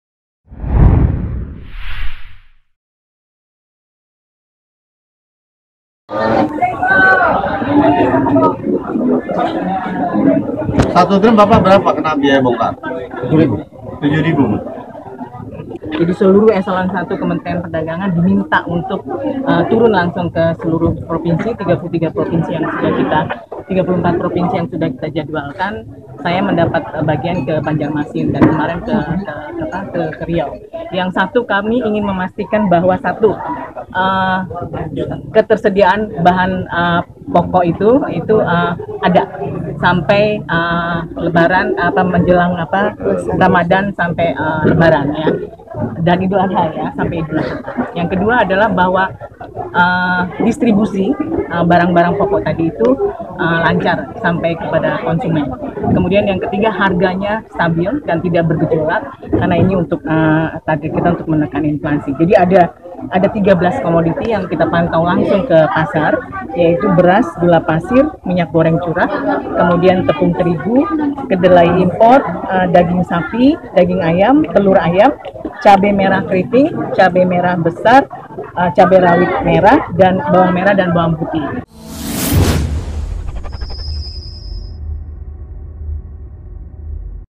Kementerian Perdagangan Pantau Harga Bahan Pokok di Banjarmasin
BANJARMASINPOST.COM.ID, BANJARMASIN- Pemerintah Provinsi Kalimantan Selatan (Kalsel) bersama rombongan Kementrian Perdagangan Republik Indonesia melaksanakan inspeksi mendadak (sidak) ke Pasar Sentra Antasari Jalan Pangeran Antasari Banjarmasin, Kamis (27/4/2017).

Gubernur Kalsel H Sahbirin Noor dan Dirjen Pengembangan Ekspor Nasional Kementrian Perdagangan Republik Indonesia Arlinda dan Kepala Dinas Perdagangan Provinsi Kalsel Birhasani langsung turun pada inspeksi pasar ini.

Inspeksi bertujuan untuk mengetahui harga pasar dan mengetahui kesiapan pasokan bahan pokok menjelang datangnya Bulan Ramadhan.

"Di Pasar Sentra Antasari utamanya kita pantau 13 macam bahan pokok termasuk daging sapi, ayam, beras, gula, cabai, kedelai dan lain-lain. Didapati tidak ada lonjakan harga dan pasokan dari distributor tidak ada masalah," kata Arlinda.(Banjarmasin Post/Achmad Maudhody)